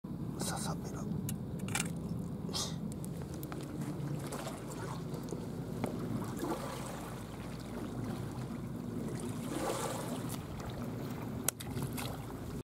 ササベラも水槽でたくさん飼育しているので、今回はキャッチ＆リリース。疑似針に沖アミを刺し餌に使えば、和歌山南紀地方ではサビキ仕掛けで炊くさ釣れます。
容姿の割に歯が鋭い肉食魚。

食用としては身が柔らかいいのですが、繊維は細かく、煮付けや塩焼きに適しています。